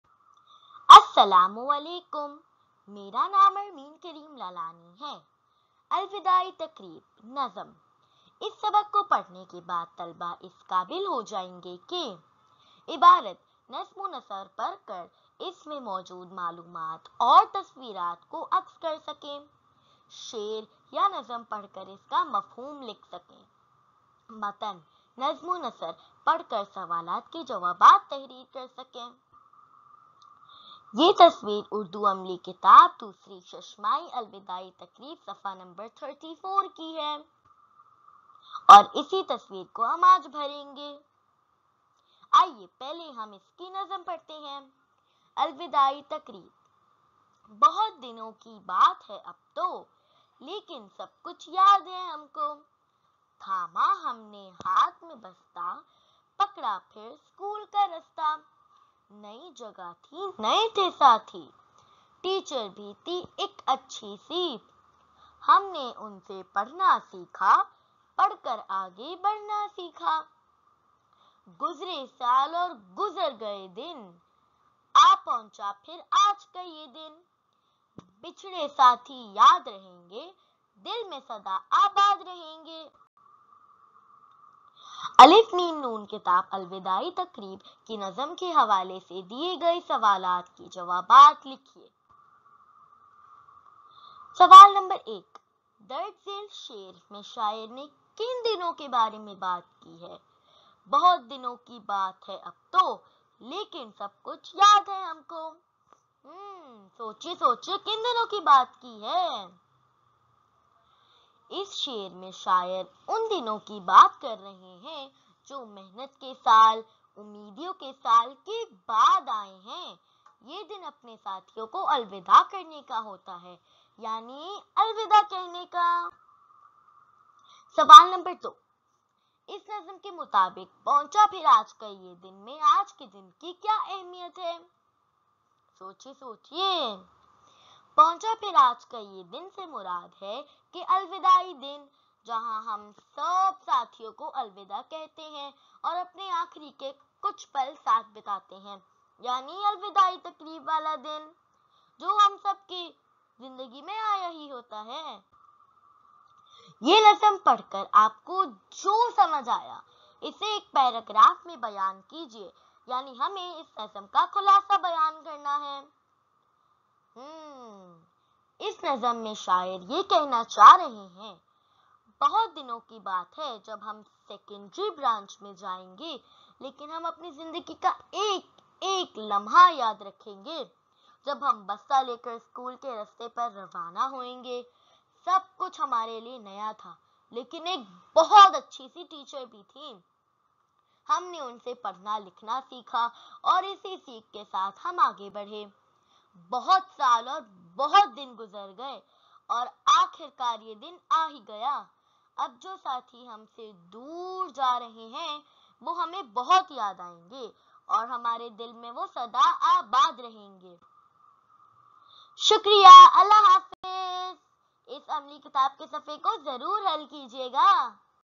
Class 5 Urdu Reader: Alwidai Taqreeb Beaconhouse School system Term 2 Urdu Workbook Page 34
Class 5 Urdu Reader: Alwidai Taqreeb
Beaconhouse School system Term 2 Urdu Workbook Page 34
Lets reinforce the topic and get ready for the exams.